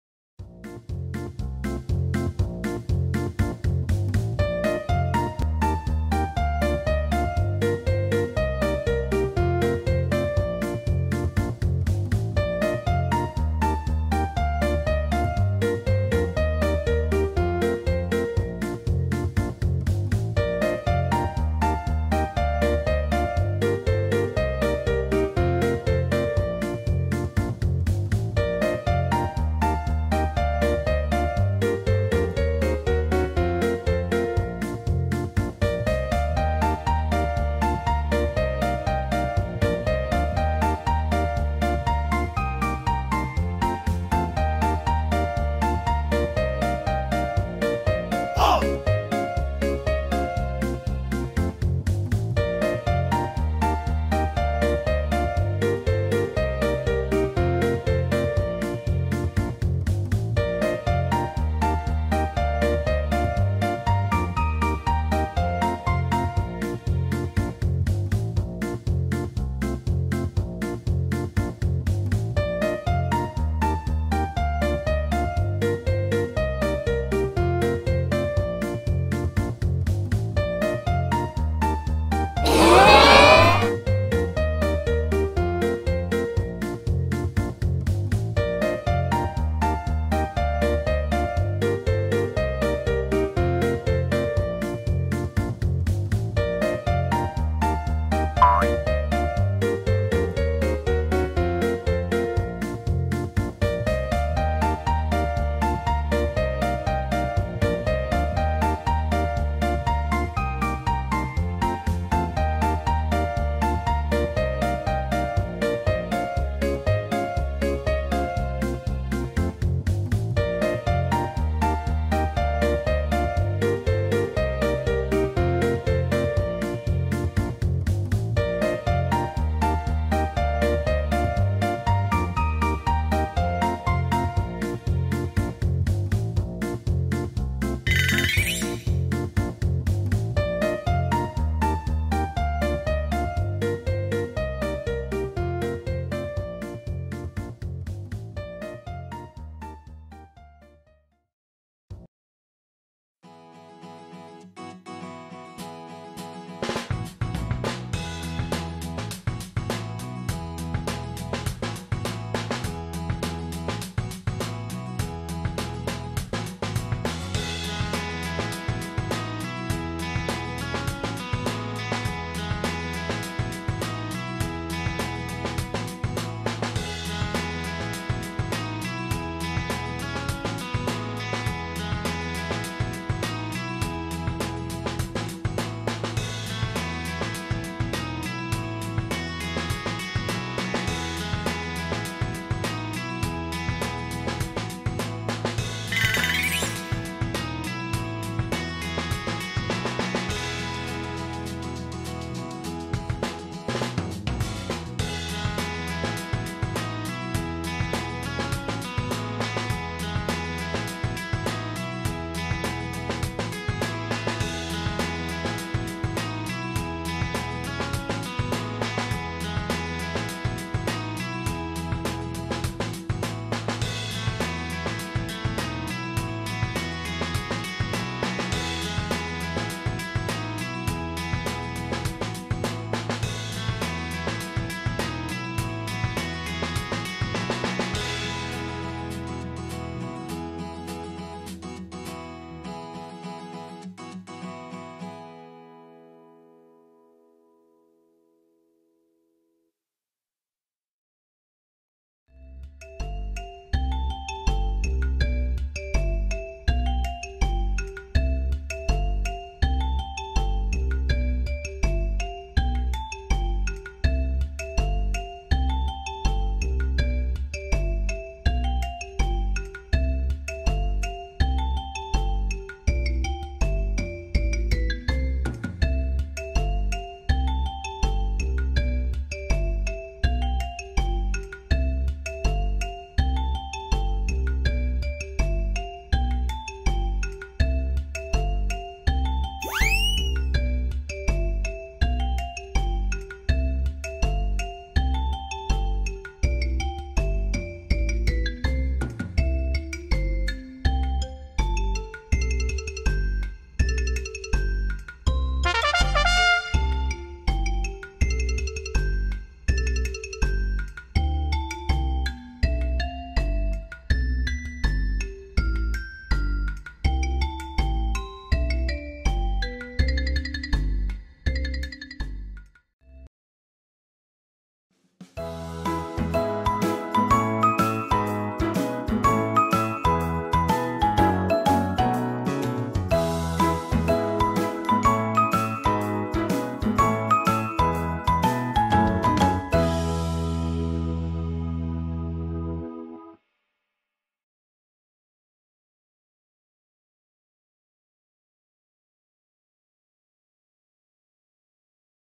うさぎと一緒に宝探しで遊んでみたら、意外な結果でおもしろかった。
ベランダでうさぎのテトくんと宝探しをして遊んでみました。すると、意外な展開に.
小さいテトくんのおもしろくて可愛い表情や行動に注目して是非ご覧ください。♪